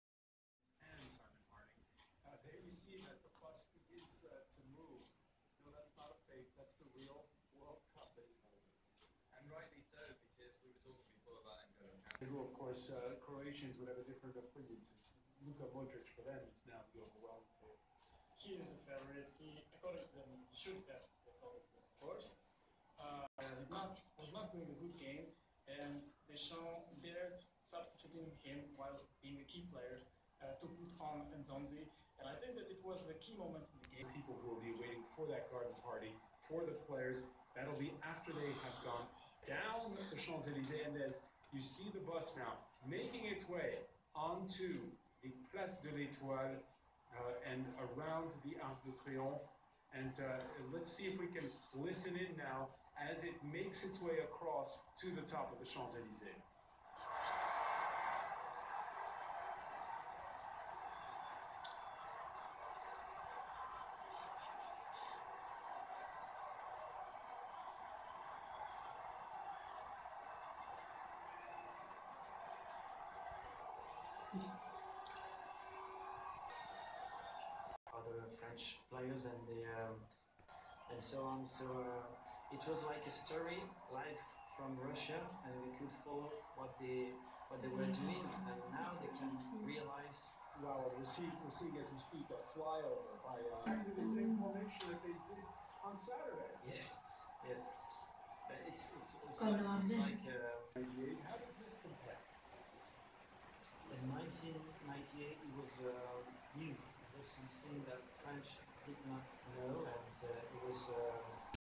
The World Cup Champions
A HERO WELCOME HOME - France The Champions of 2018 World Cup